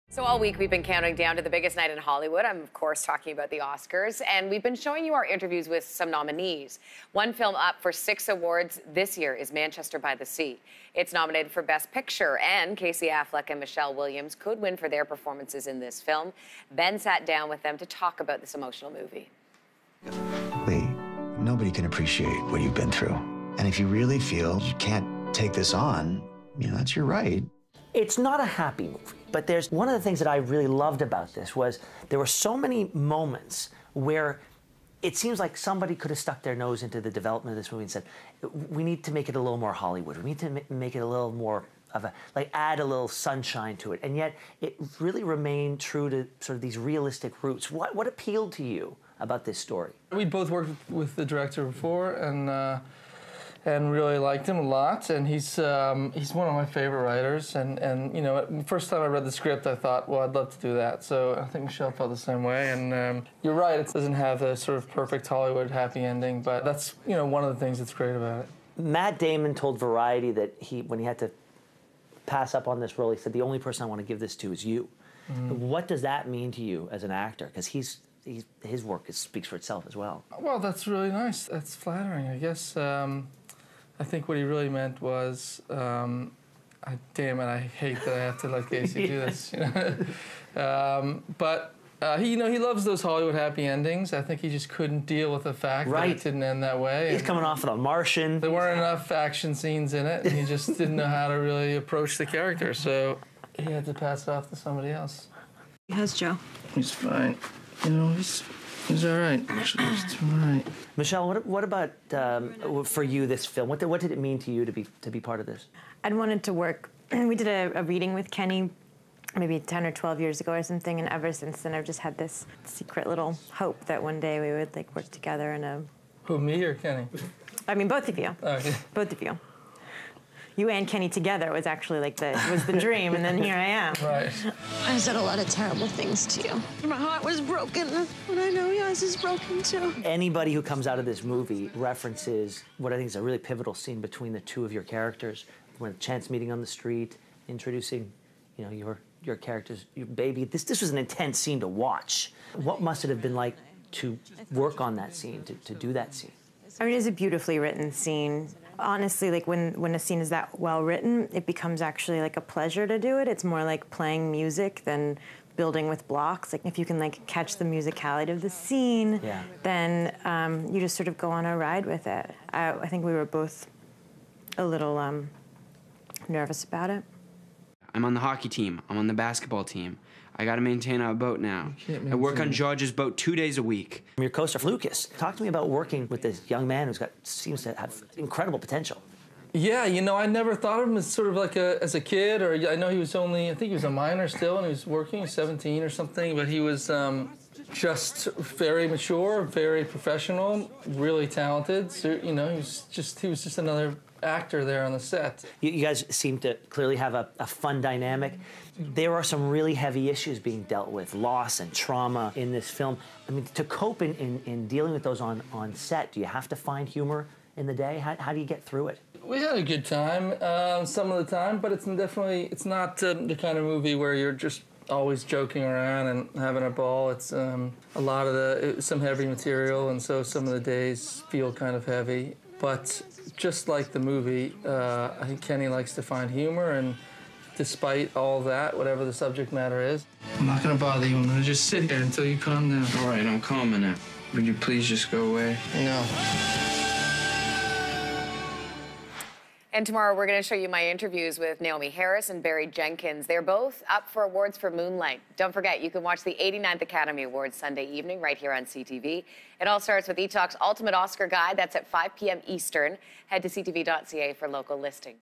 Casey Affleck on the non-typical ending in 'Manchester by the Sea' | Your Morning
Ben sits down with Casey Affleck and Michelle Williams to talk about 'Manchester by the Sea' and what sets it apart.